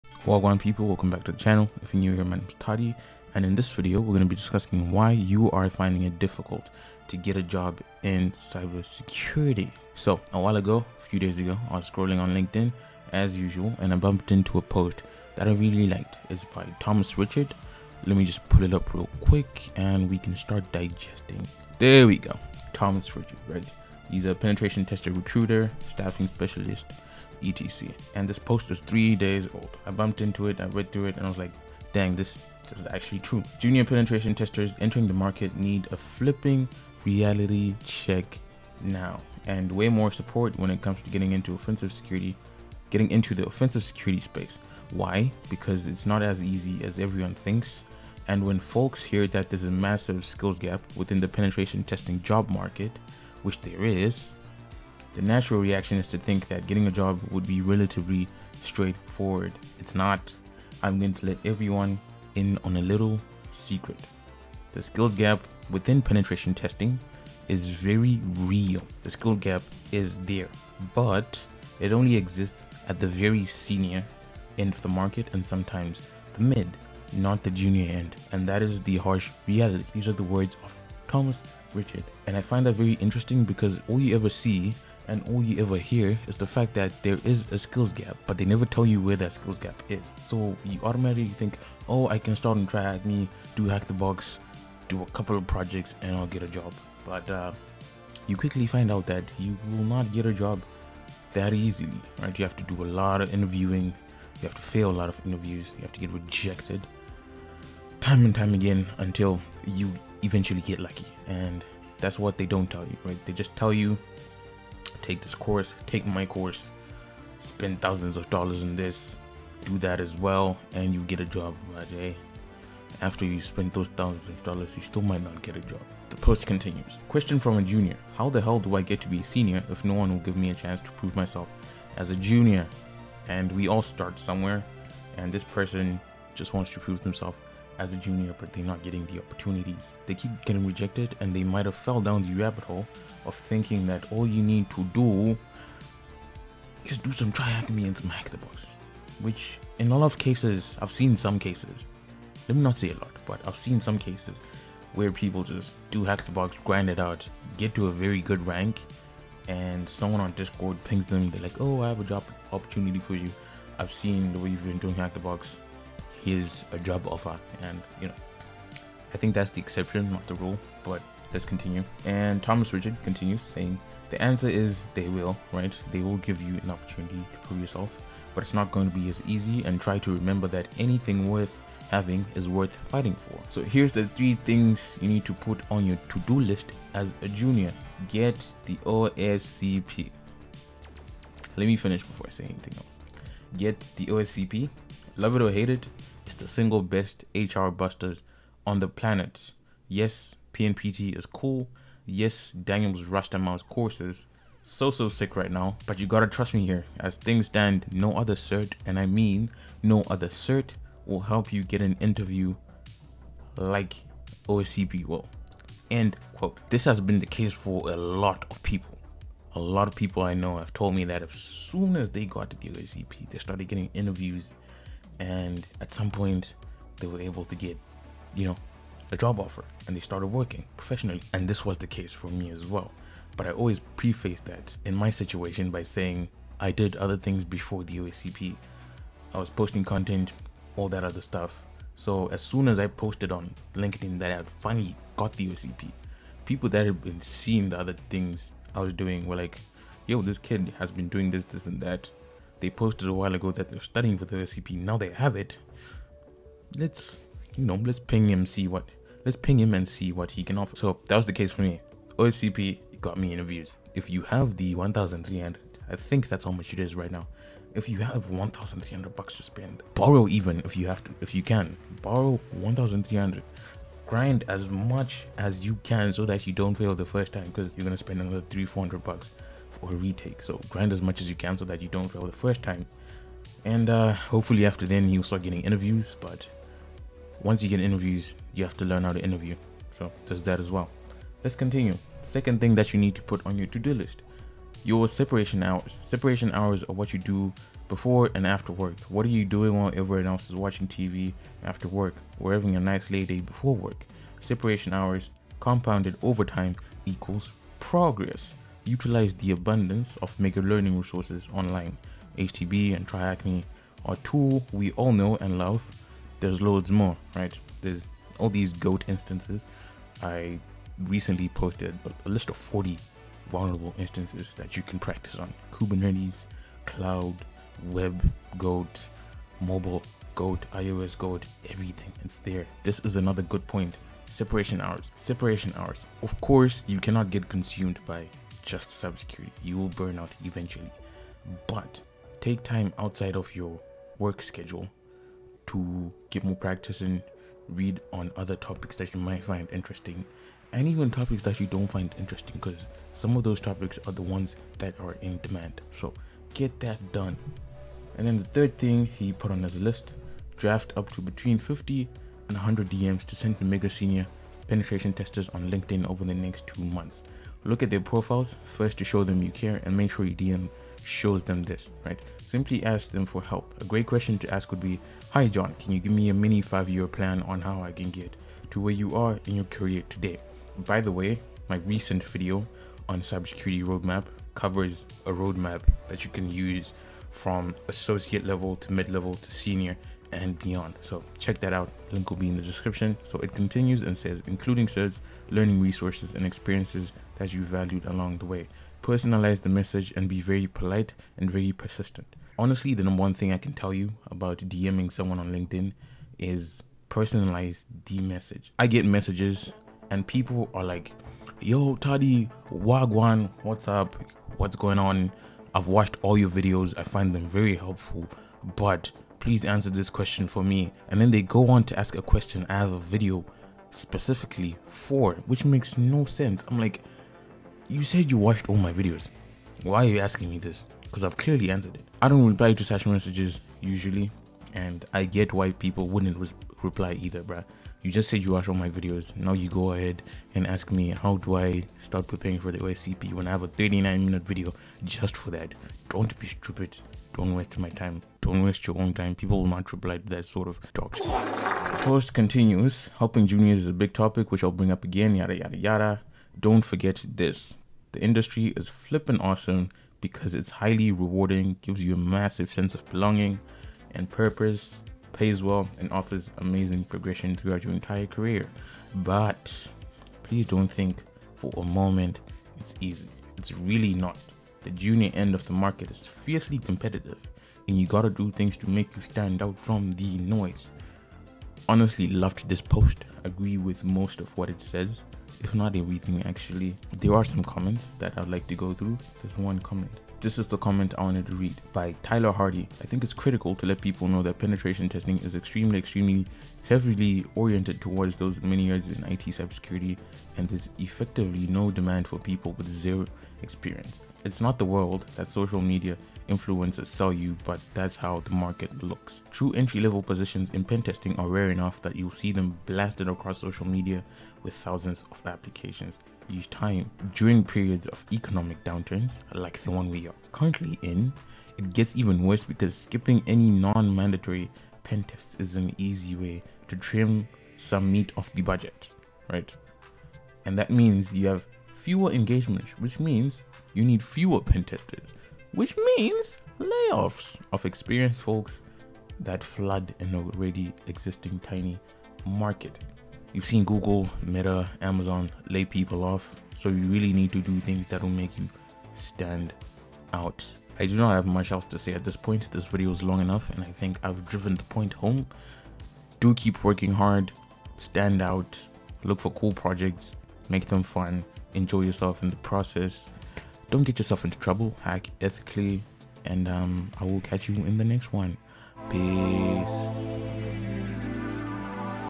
Why You Can't Get A Job In Cybersecurity
Another one of my random opinion videos discussing some things aspiring penetration testers should put on their to do list. These include:
- Getting the OSCP,
- Getting in practice during down time,
- Reaching out to seniors on LinkedIn with reasonable questions.